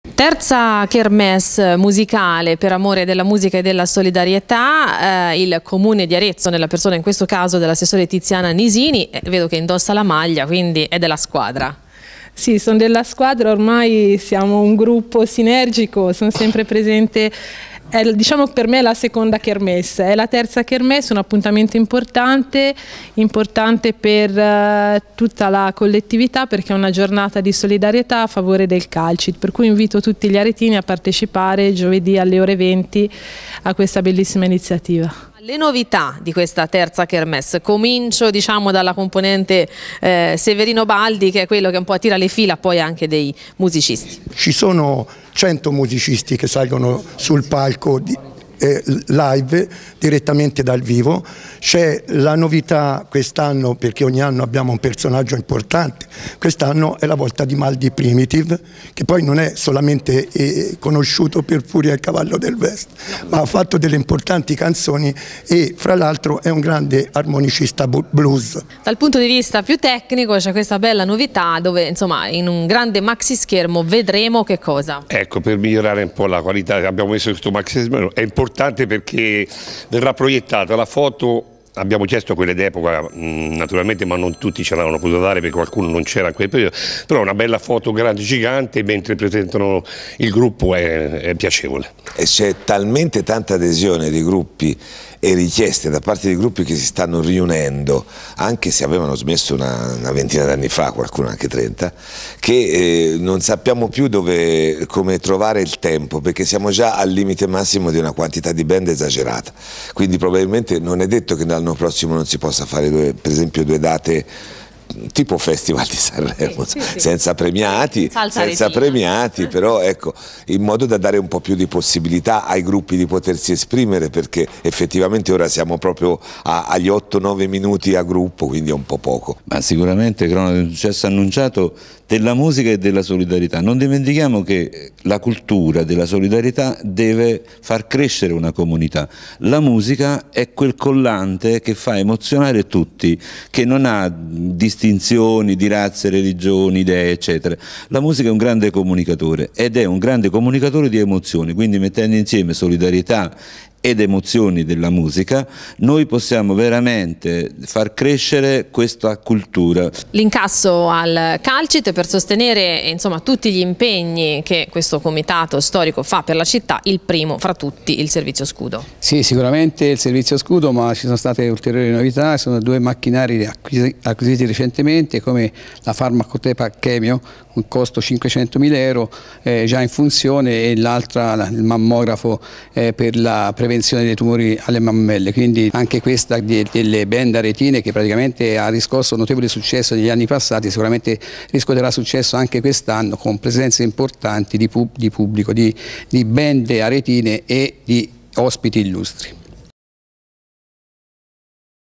Per amore della musica e della solidarietà, tornano le storiche band aretine
Giovedi al teatro Tenda in favore del Calcit